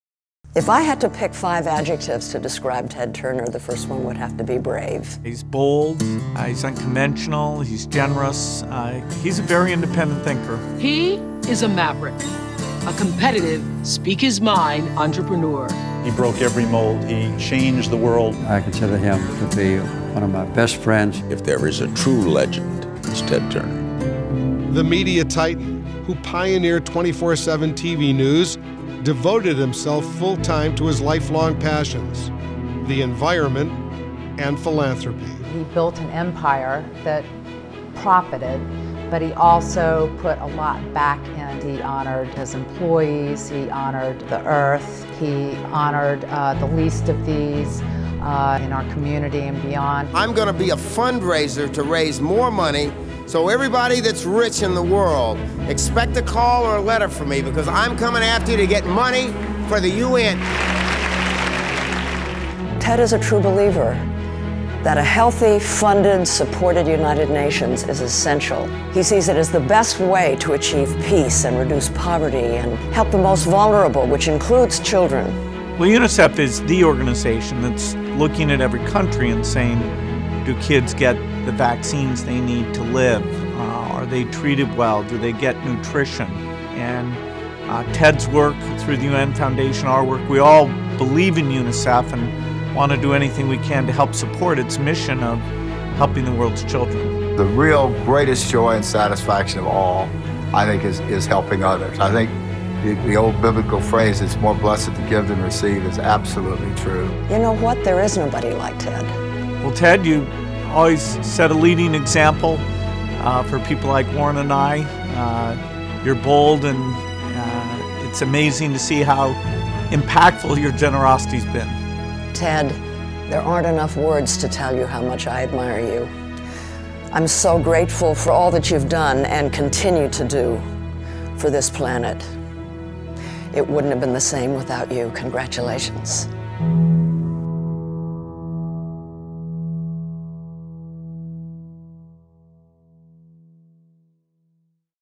Ted Turner: 2016 Global Philanthropist Award (UNICEF)
Ted, Turner, Founder of the United Nations Foundation, was honored with the 2016 U.S. Fund for UNICEF Global Philanthropist Award.  Ted received the award at the second annual Evening for Children First event in Atlanta. The award honors him for his work on behalf of children everywhere. When Ted accepted the award, he said he knew that when he gave $1 billion, it was one of the most important decisions he’d ever make.